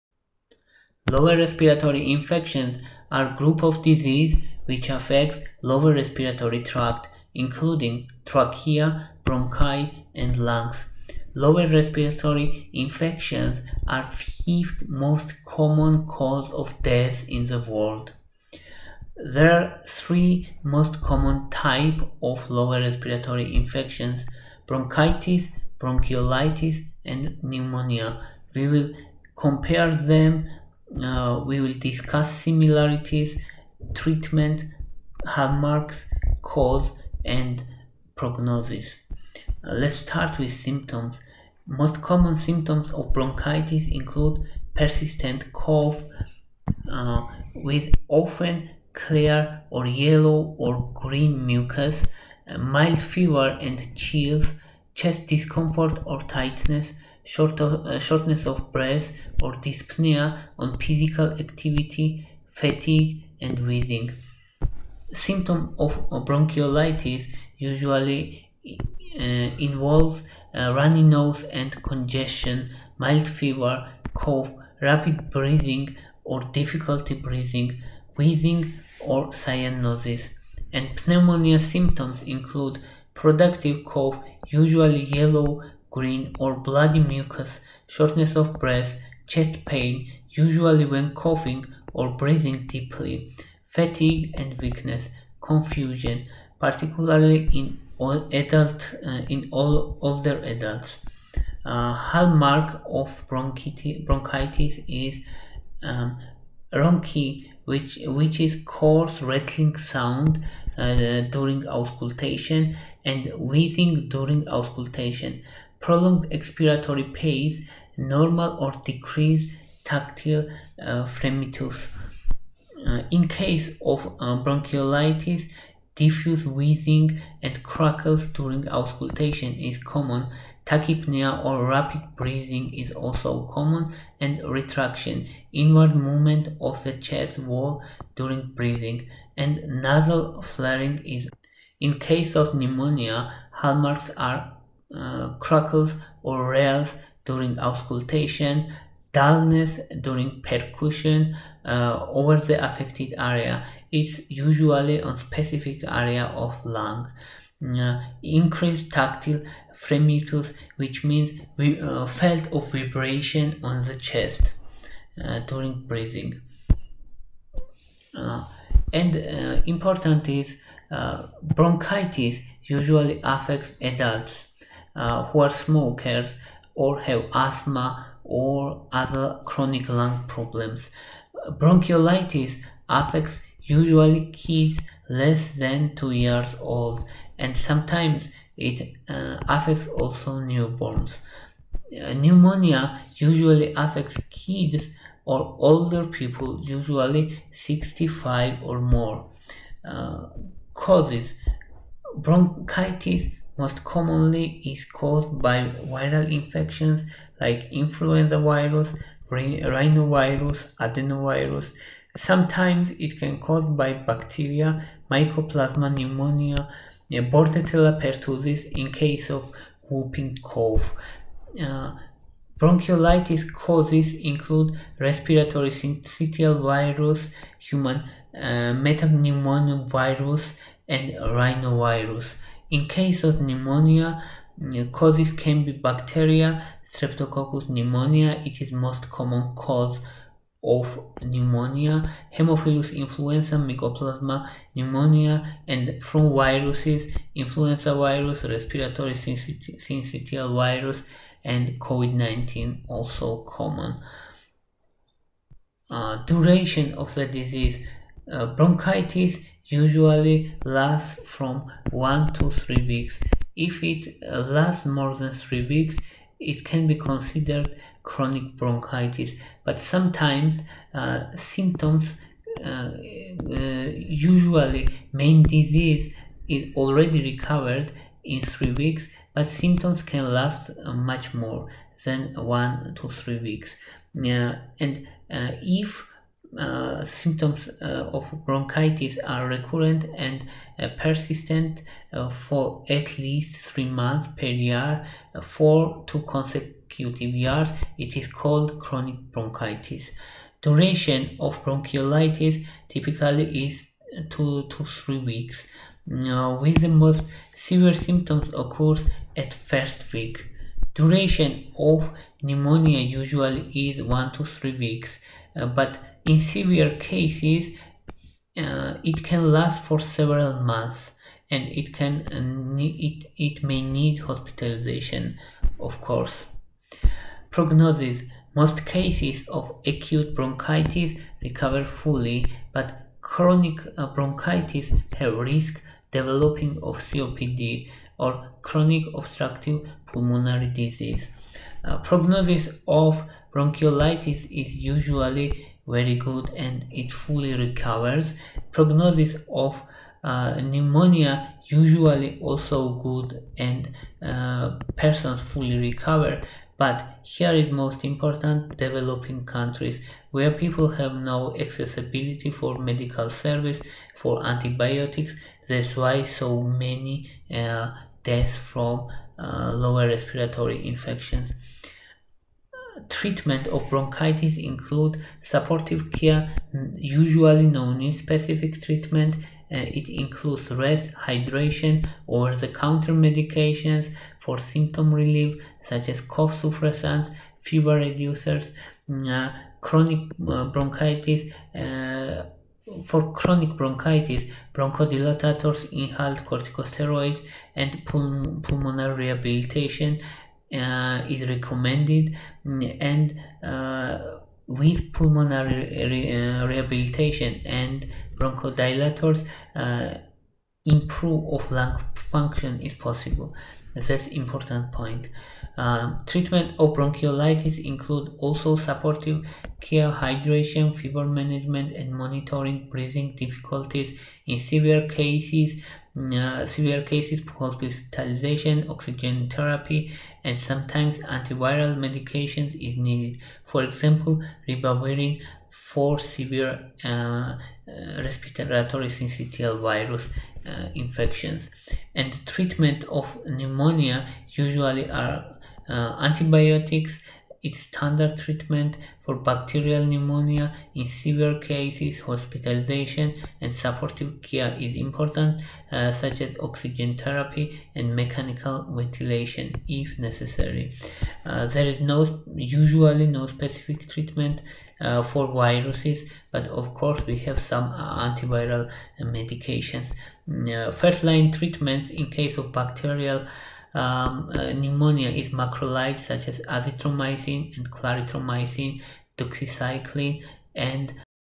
Lower Respiratory Infections: Bronchitis, Pneumonia, Bronchiolitis.
Bronchitis vs Pneumonia vs Bronchiolitis.
What is the duration of bronchiolitis?
What is the standard treatment for pneumonia?
What is the long-term prognosis for chronic bronchitis?
Is full recovery possible from bronchiolitis?
What are the common symptoms of community-acquired pneumonia?
How long does acute bronchitis typically last?
What are the possible complications of pneumonia?
What is the main treatment for bronchiolitis?
What are the potential long-term respiratory problems of severe bronchiolitis?
What is the difference between acute and chronic bronchitis?

By Matteo Di Nardo, Daniela Perrotta, Francesca Stoppa, Corrado Cecchetti, Marco Marano and Nicola Pirozzi - Independent lung ventilation in a newborn with asymmetric acute lung injury due to respiratory syncytial virus: a case report, CC BY 2.0,